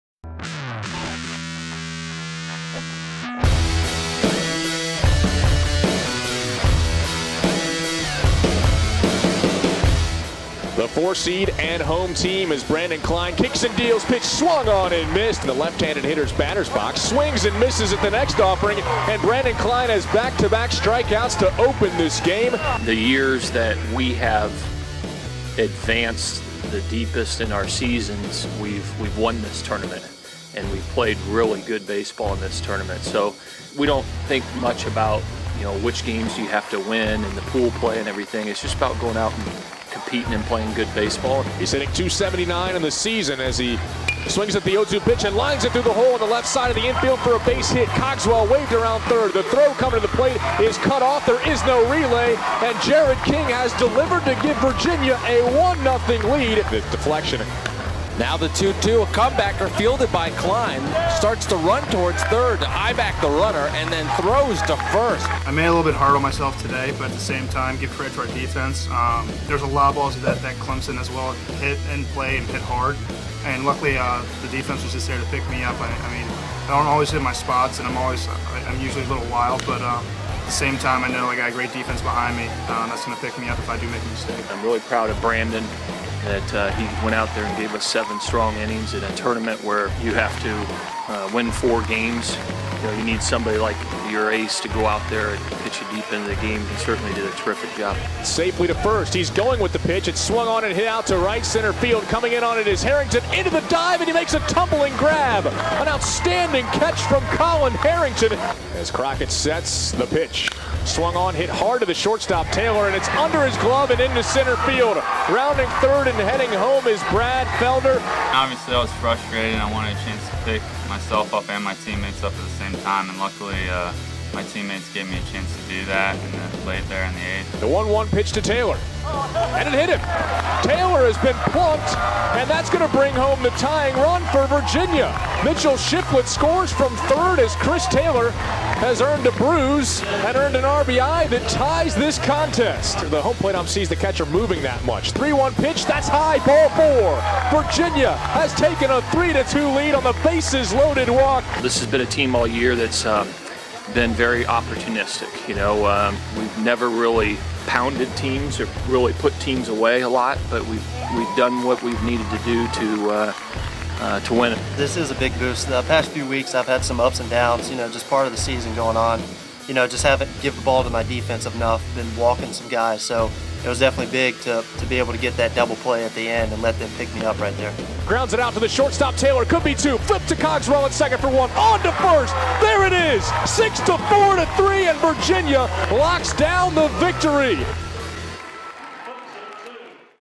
Baseball Highlights - Clemson ACC Tournament
No. 17 Cavaliers Rally to Top Clemson, 3-2, in ACC Opener.